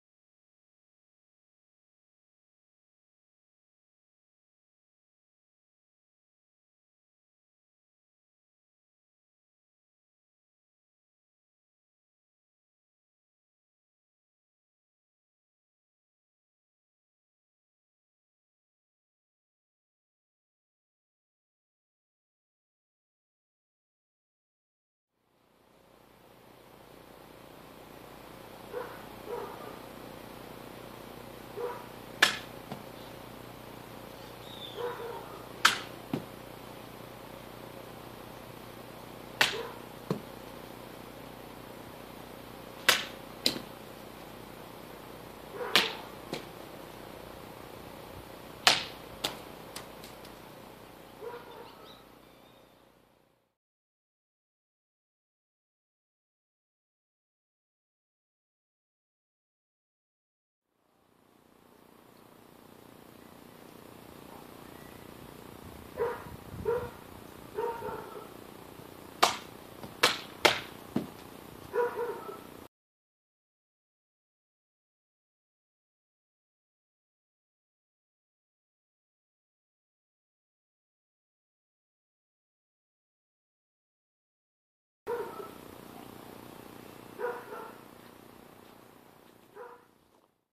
Cutting Tatami with the Windlass Arbedo sword
Today, I cut Mugen Dachi tatami mats with a Windlass Arbedo. I ordered the sword unsharpened and spent a couple of hours sharpening the sword before cutting. As can be seen in the video, the sword cut the mats very well.